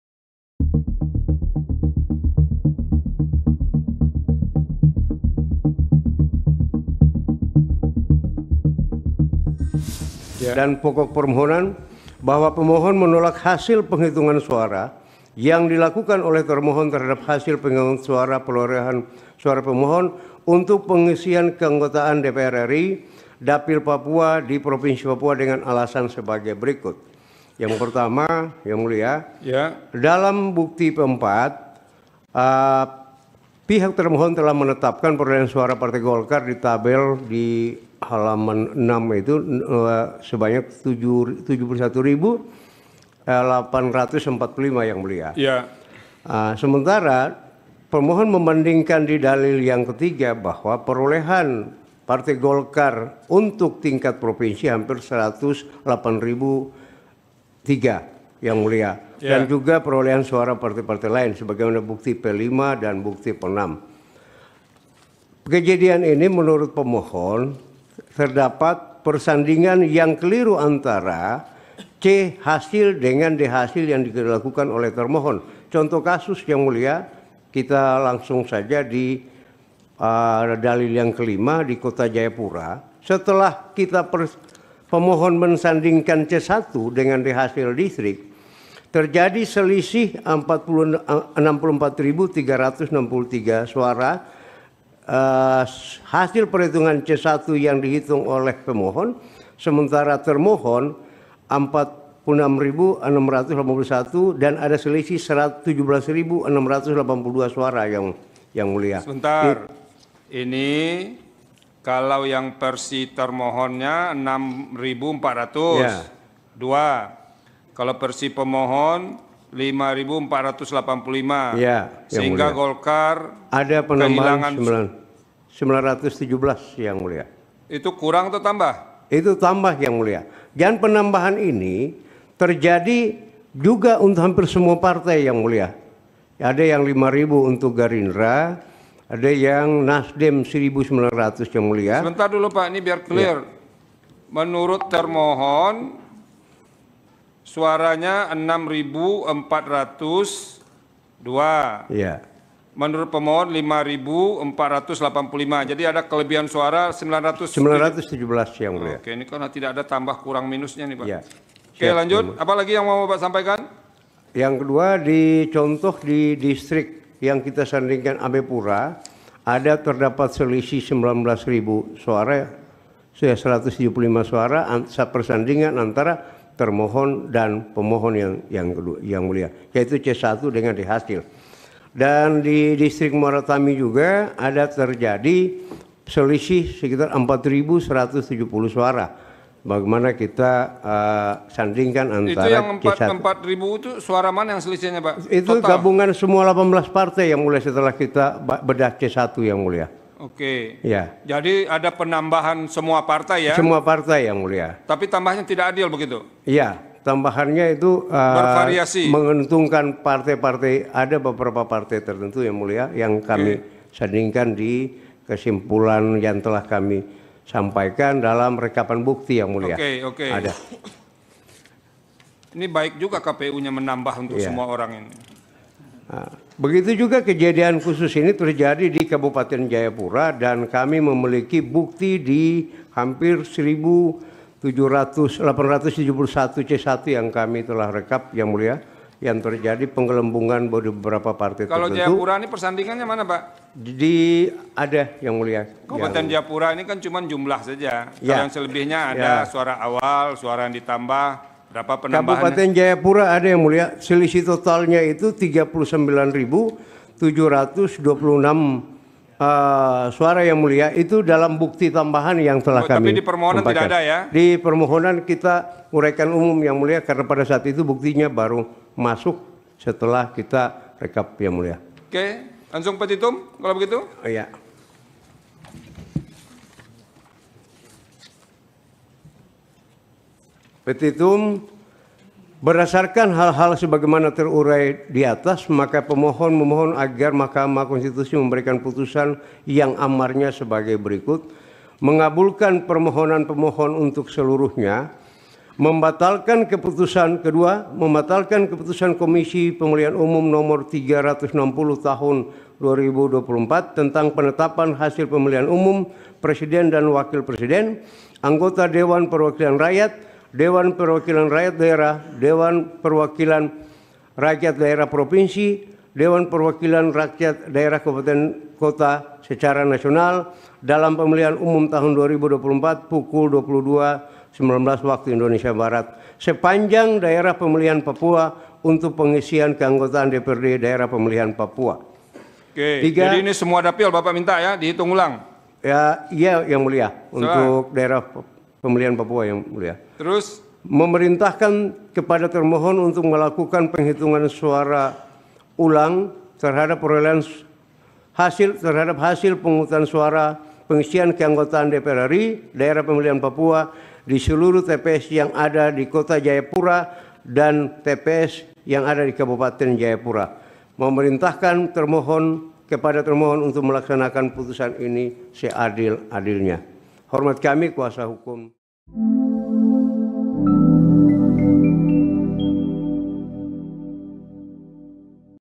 SEBUT HASIL PILEG PAPUA ANOMALI, GOLKAR MINTA PENGHITUNGAN ULANG
Partai Golongan Karya (Golkar) menolak Surat Keputusan Komisi Pemilihan Umum (KPU) Nomor 360 terhadap hasil penghitungan perolehan suara untuk keanggotaan DPR RI daerah pemilihan (dapil) Papua.

YouTube Forum Keadilan TV
FB forumkeadilandotcom
Tiktok forumkeadilantv
Helo forum.keadilan